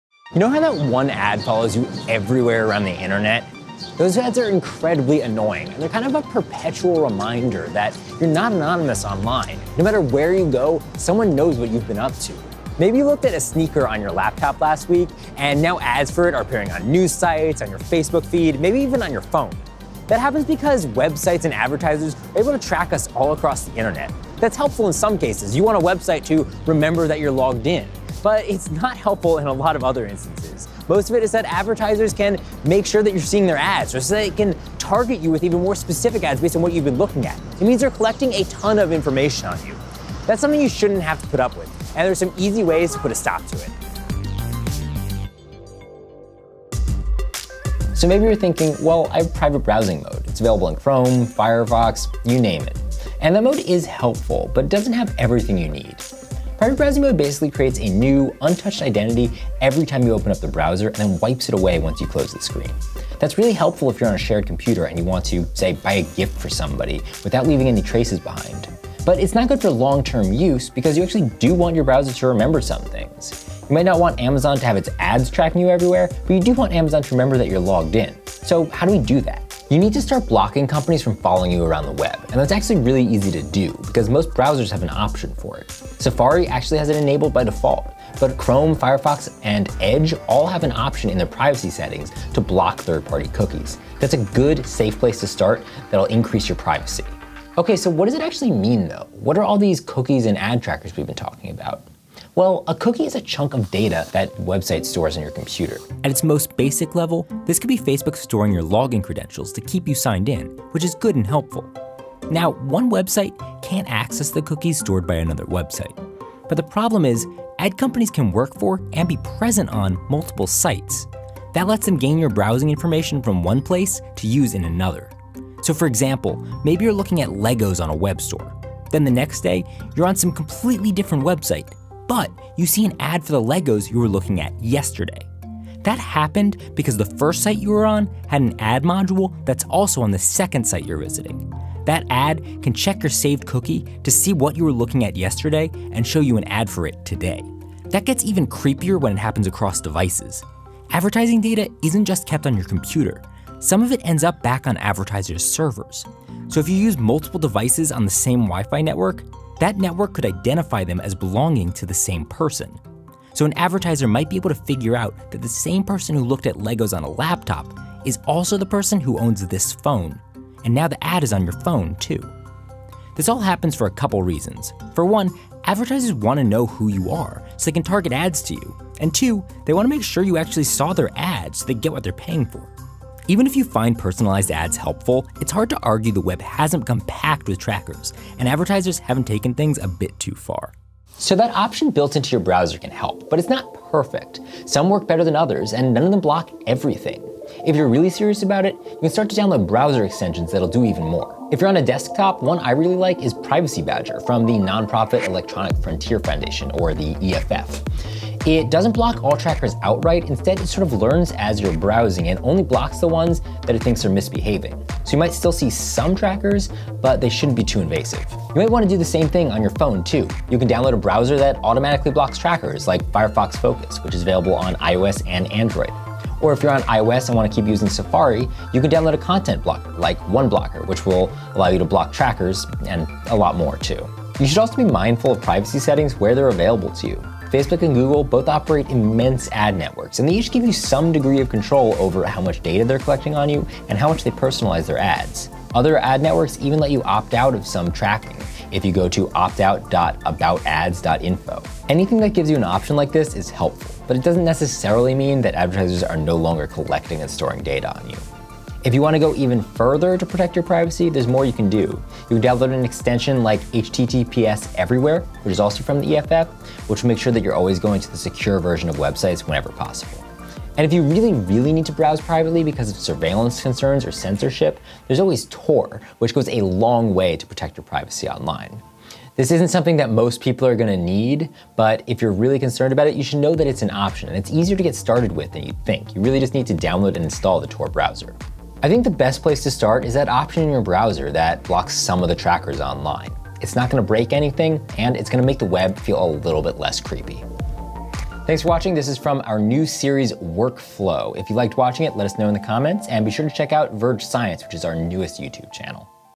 These tools can protect your online privacy
You know how that one ad tracks you everywhere online? Here's how to get rid of it, and a bunch of other things you can do to improve your privacy online.